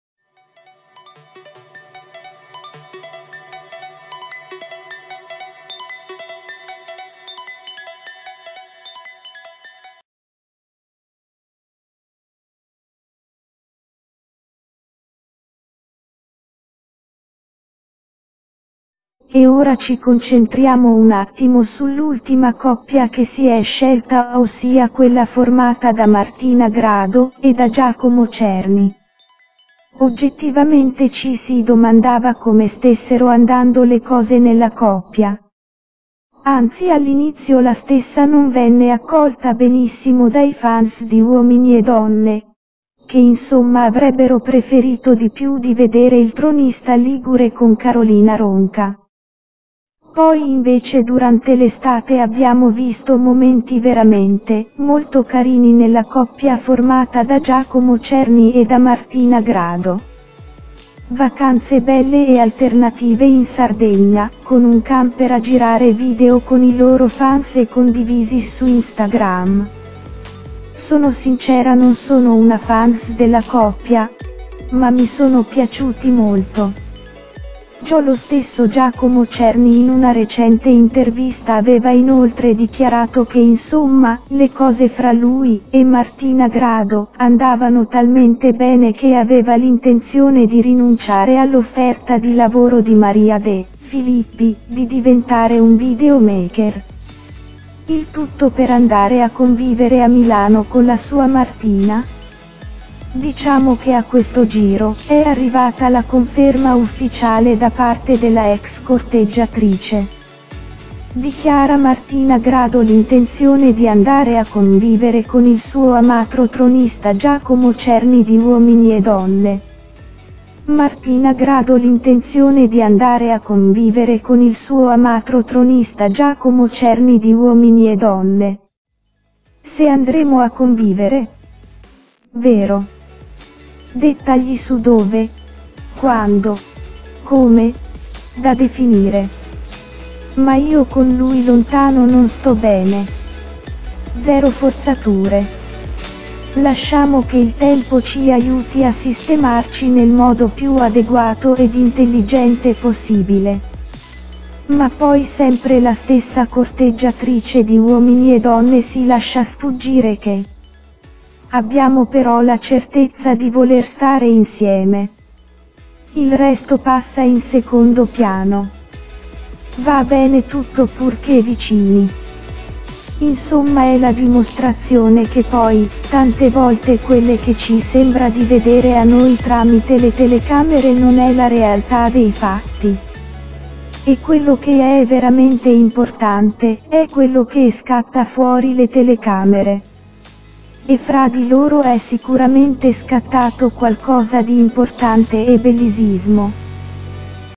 Uominiedonne Martina Grado e Giacomo Czerny vanno a convivere
E adesso dedichiamo questo articolo video a Martina Grado e Giacomo Czerny ci sono grosse novità che riguardano la coppia. Ossia finalmente hanno preso la grande decisione. Da Settembre andranno a convivere. Di fatto se ricordate nemmeno troppo fra le righe l'avevano fatto capire. Ovviamente dopo un'estate passata insieme dove di fatto hanno già convissuto.